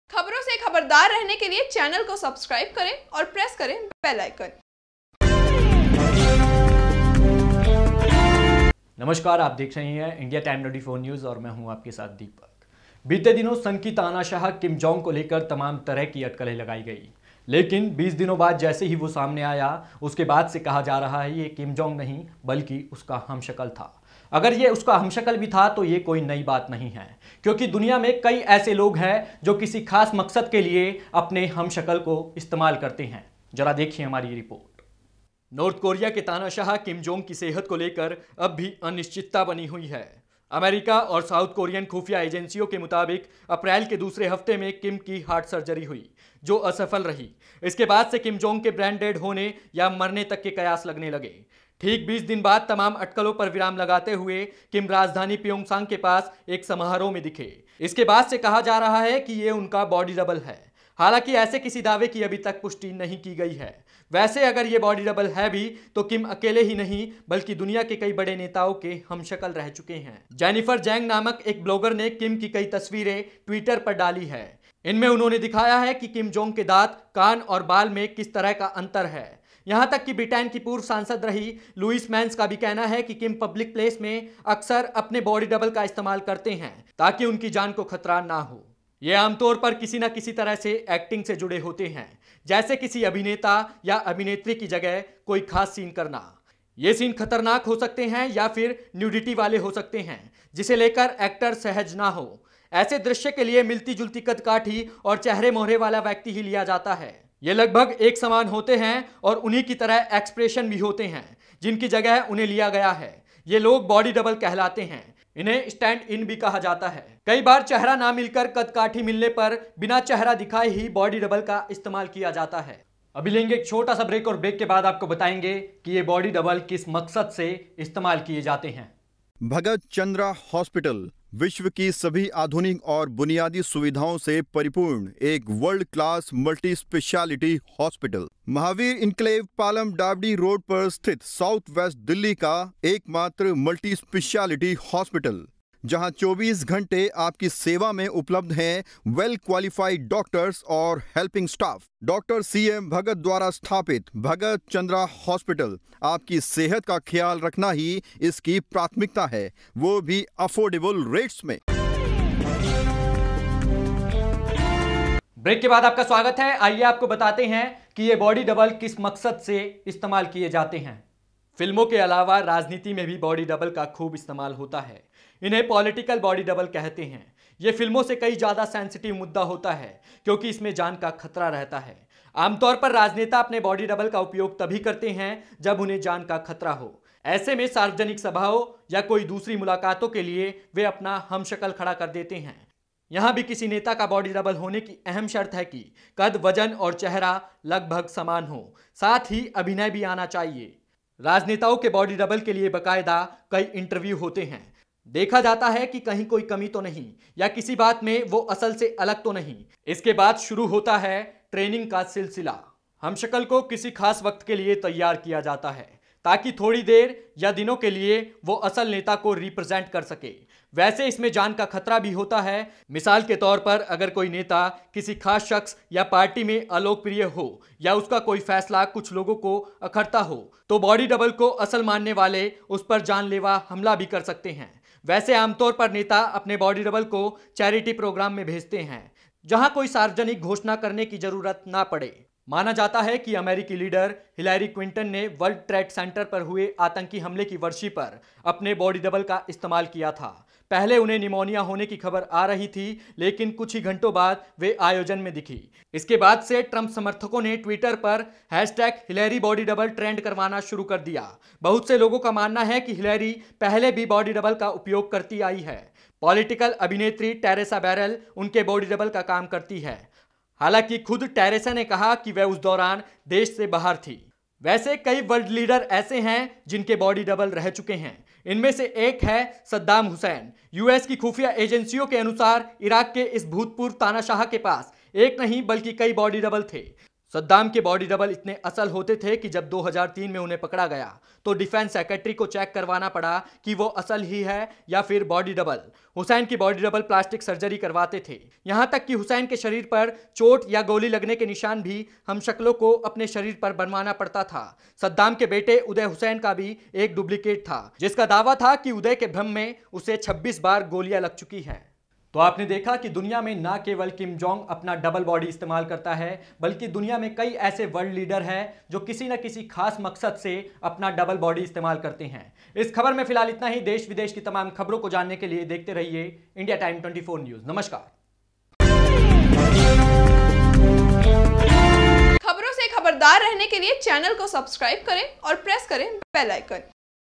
जो 20 दिनों बाद दिखा वो Kim Jong Un नहीं बल्कि था उसका हमशक्ल, देखिए
बीते दिनों दुनियाभर में अपनी हैवानियत के लिए जाना जाने वाले तानाशाह किमजोंग को लेकर तमाम तरह की अटकलें लगाई गई हैं। लेकिन 20 दिनों बाद जैसे किम जोग सामने आया उसके बाद से कहा जा रहा है कि ये किमजोंग नहीं बल्कि उनका हमशक्ल था। अगर ऐसा भी है तो ये कोई नहीं बात नहीं, क्योंकि दुनिया में कई ऐसे लोग है, जो अपने किसी खास मकसद के लिए अपने बॉडी डबल का इस्तेमाल करते हैं।